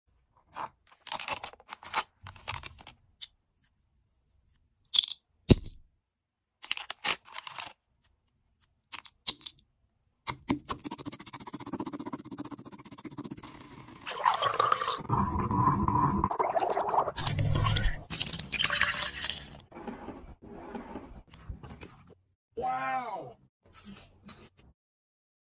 Satisfying crushing, DIY ice cream.
DIY ice cream, use candy to make ice cream.